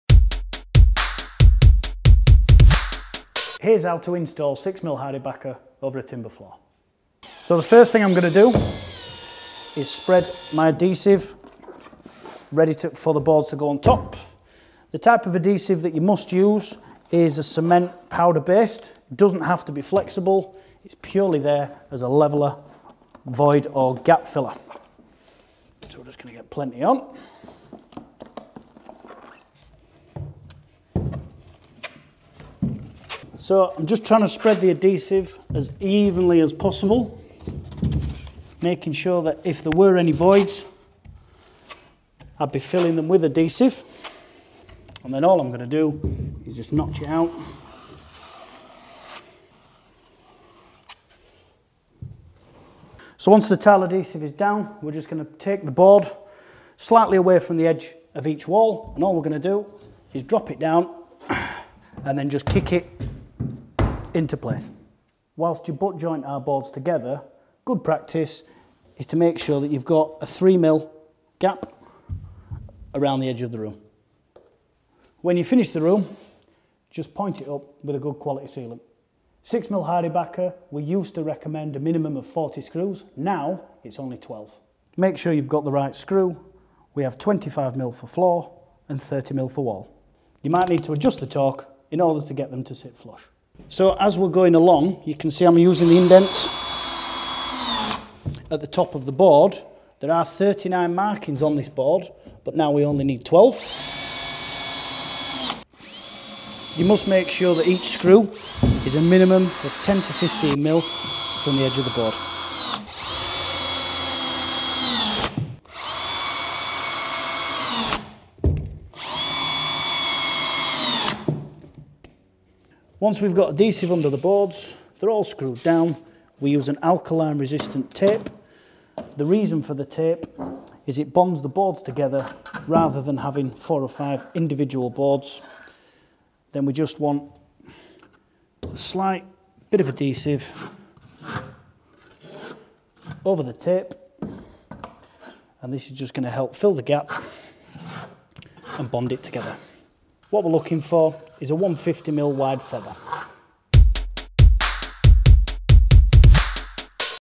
How to: Installing Hardie® Backer 6mm fibre cement tiling backerboard onto a timber or wooden floor
This instruction video shows how to install 6mm Hardie® Backer fibre cement backerboard onto a wooden or timber floor.

Hardie® Backer is a cement tiling backerboard especially suited to installation as a tiling base for floors in wet areas: Toilets, Bathrooms, Kitchens etc.

Hardie® Backer is often specified as a tiling backerboard as an alternative to plasterboard, plywood and chipboard as it requires no priming or any other treatment as a tiling backerboard in wet areas.

Hardie® Backer backerboard is
- Water resistant
- Mould resistant
- Fire resistant / Non-combustible